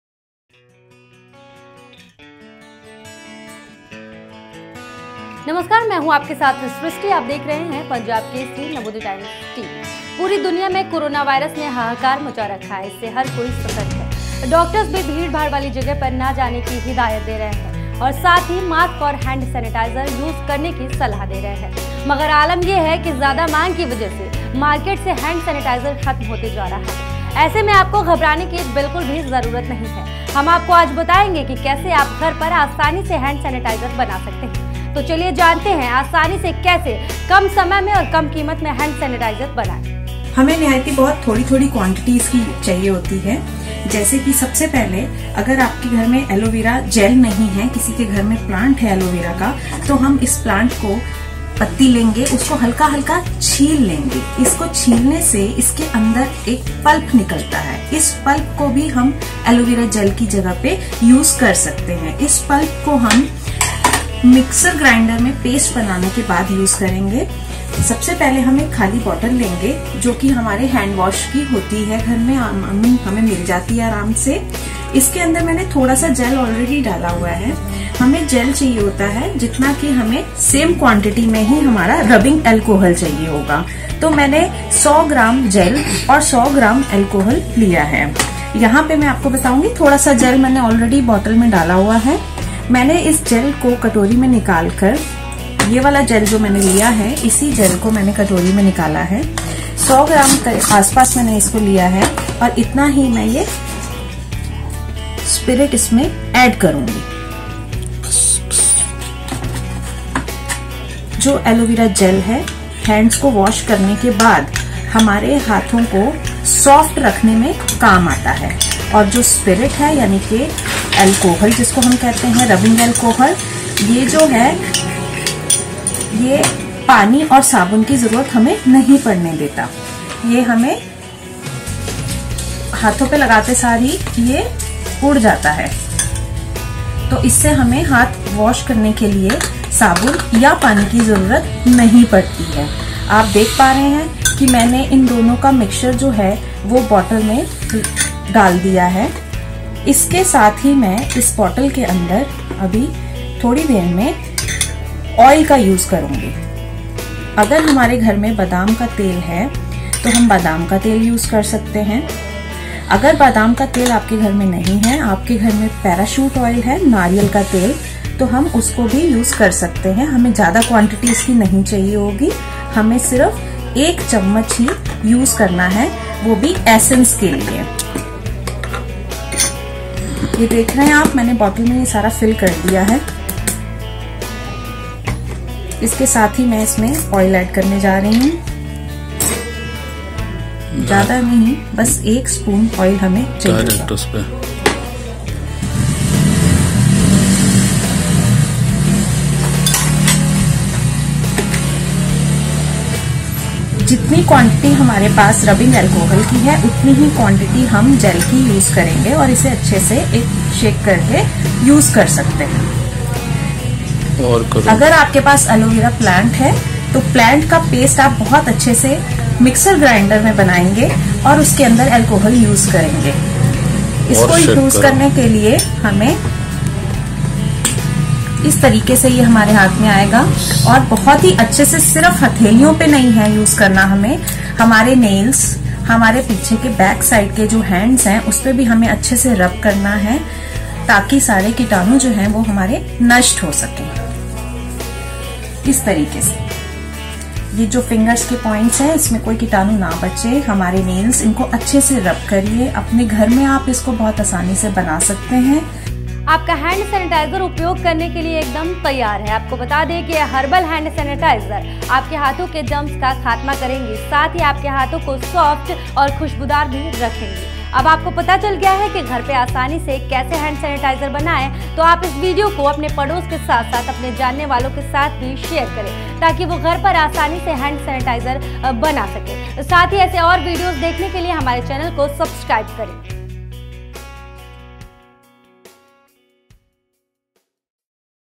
कम समय और कम कीमत में आसानी से घर पर बनाएं हैंड सैनिटाइजर
आपका हैंड सैनिटाइजर उपयोग करने के लिए तैयार है.. आपको बता दें कि
ये हर्बल हैंड सैनिटाइजर हाथों के जर्म्स् का खात्मा  करेगी। इतना ही नहीं इसे लगाने से हाथ भी बिल्कुेल ड्राई नहीं होंगे। आप इसे आराम से अपनी पॉकेट में या पर्स में कैरी कर सकती हैं। यह हाथों पर बेहद सॉफ्ट है और खुशबूदार है.. तो आप अब खुद घर पर अपने हाथों से इसे बनाइये और आपके पड़ोस के साथ-साथ जानने वालों के साथ भी इस वीडियो को शेयर करें .. जिससे वो भी इसे आसानी से घर पर ही बना सकें... इस तरह के और वीडियो देखने के लिए आप हमारे चैनल को सब्सक्राइब भी करें..